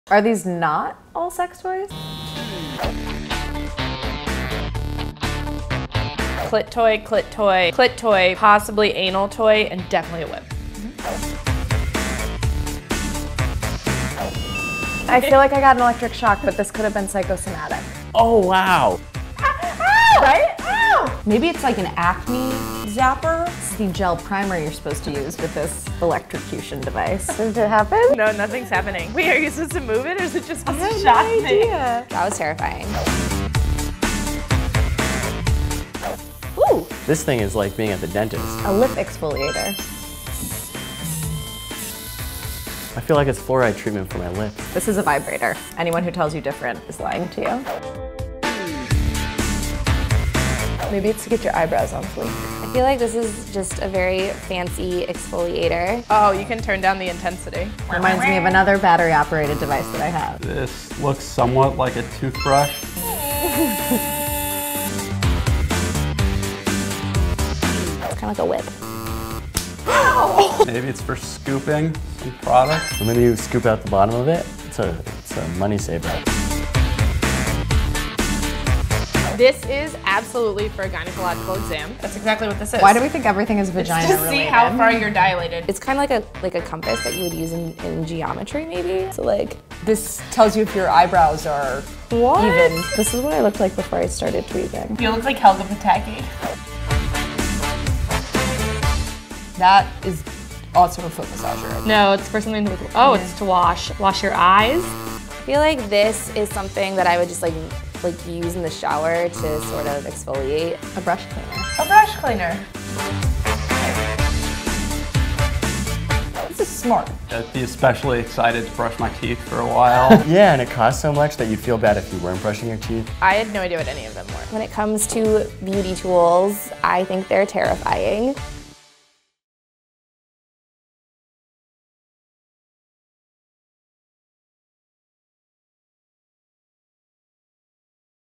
We Try Weird Beauty Tools So You Don't Have To
If someone gave you a device equipped with microcurrent shocking balls, would you know to use it on your face? These Bustle staffers tried out some weird beauty tools and couldn’t quite figure out their purpose.